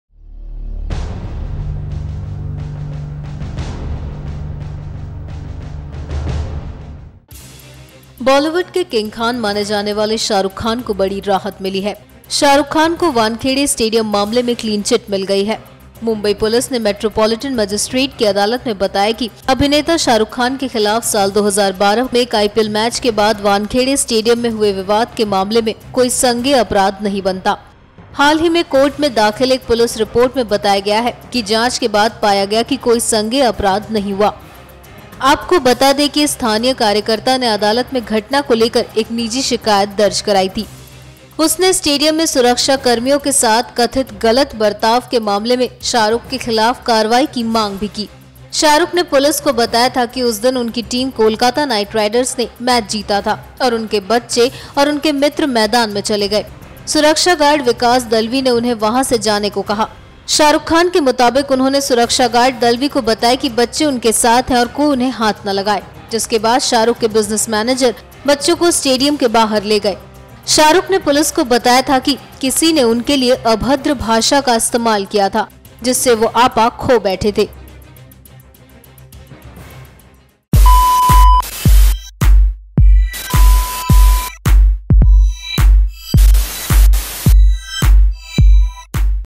Wankhede stadium case में Mumbai police ने Shahrukh Khan को दिया Clean Chit
The Mumbai police have informed a metropolitan magistrate’s court that no ‘cognisable offence’ was made out against Bollywood superstar Shah Rukh Khan in connection with a brawl at the Wankhede stadium here after an IPL match in 2012